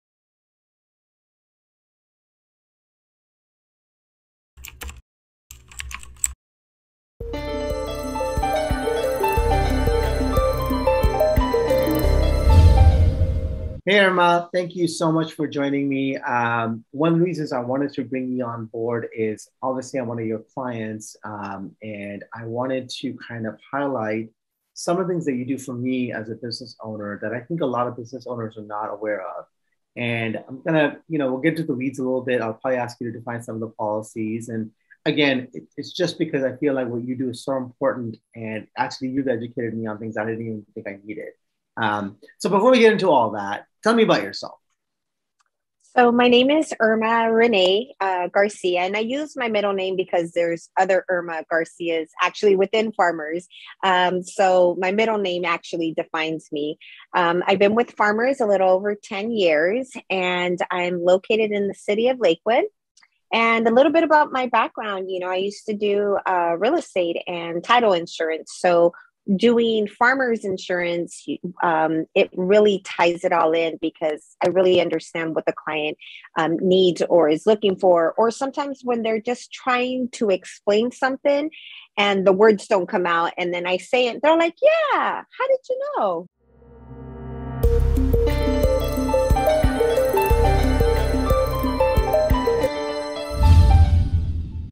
25.1. About Irma Renee
Irma Renee Garcia is the Agency Owner with Farmers Insurance Group in the city of Lakewood, since 2012.
She has received the Blue Vase and Toppers Awards through her Farmers Insurance Company. In 2021, She named Top 3 agent in Commercial Insurance for her District. Is a Founder Member of a Multi-Million Dollar Chapter – Professional Alliance Business Networking Group in the city of Cerritos. Served as the Secretary for the term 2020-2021.

Sanjay Sabarwal focuses his practice in the areas of Employment Law, Family Law and Personal Injury Law. Mr. Sabarwal has experience in Business Law, Wage Compensation, Partnership Disputes and Mediation. Mr. Sabarwal has been the Co-Owner and House Counsel for Ziba Beauty, member of the American Bar Association, Los Angeles Bar Association, South Asian Bar Association and Orange County Bar Association. He also serves on the Advisory Board for Youth Business Alliance, which connects entrepreneurs to speak at underserved schools in Los Angeles County. He is also a board member for the Artesia Chamber of Commerce.

If you have questions or need some advise on how to handle a legal situation.

Sanjay Sabarwal, Esq
Sabarwal Law